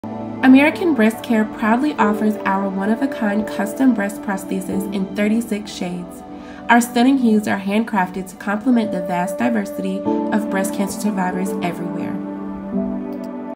Custom Breast Prosthesis- 36 Shades!
Our Custom Breast Prosthesis is not just about finding the perfect match- it's about empowering survivors to feel comfortable and confident in their own skin!

That's why we are thrilled to offer 36 shade options that reflect the diverse beauty of survivors everywhere.